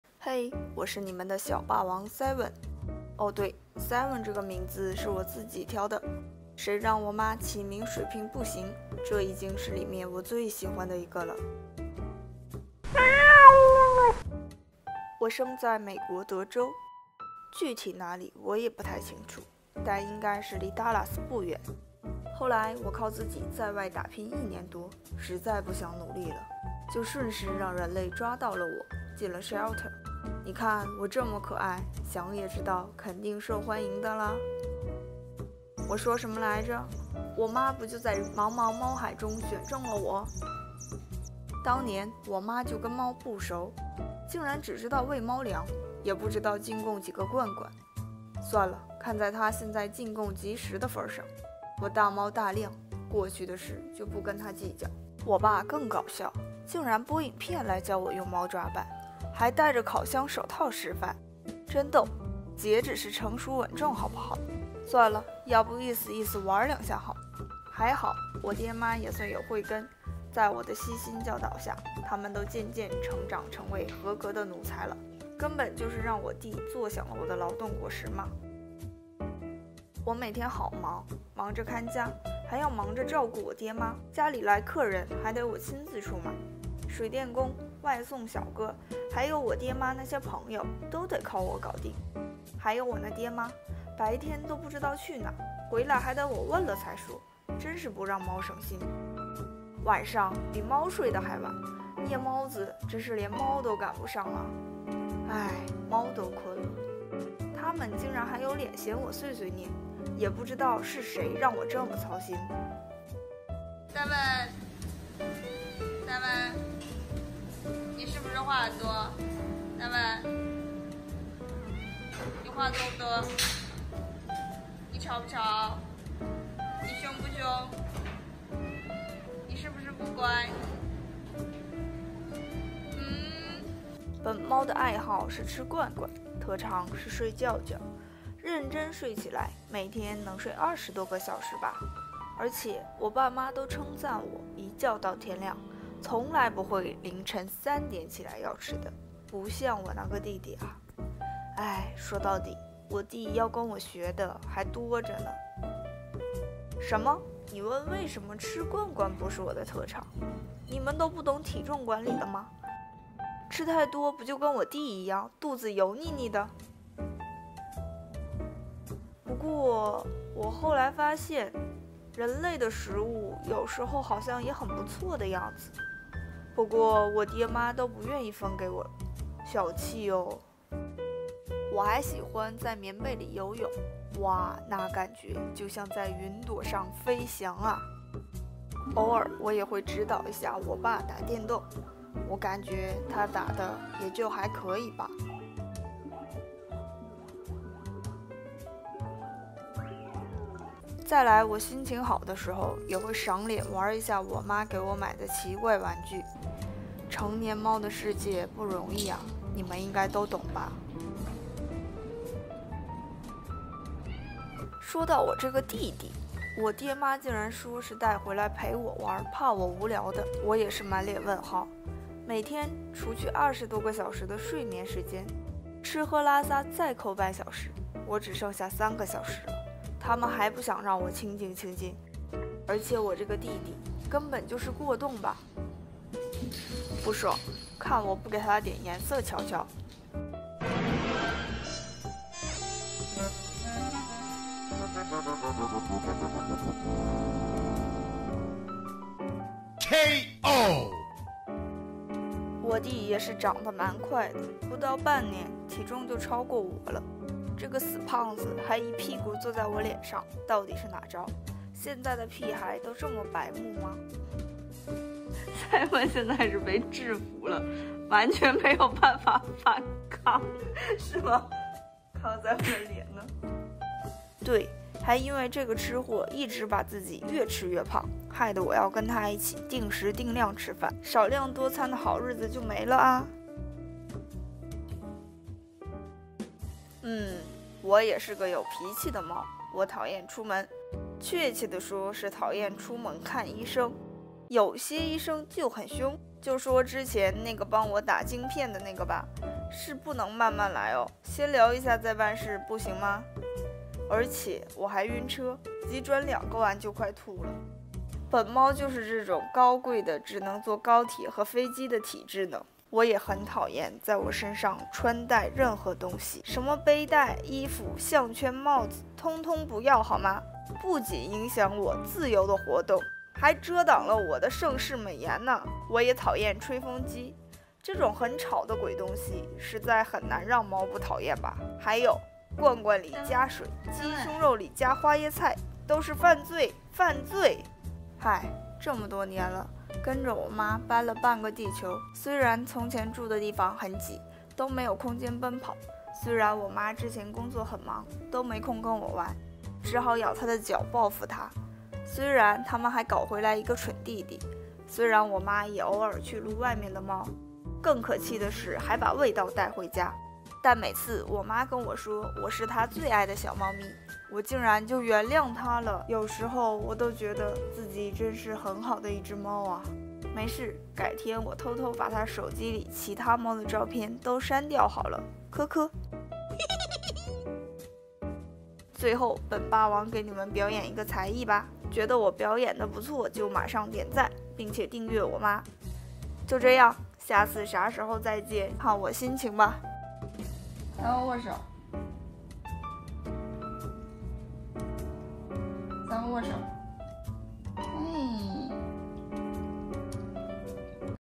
我是你們的“小霸王”Seven：一隻從美國到台灣的藍貓（懶貓？）的自白｜Cheers Vlog
hey我是Seven，是個溫暖的小霸王，希望你們喜歡我的故事❤️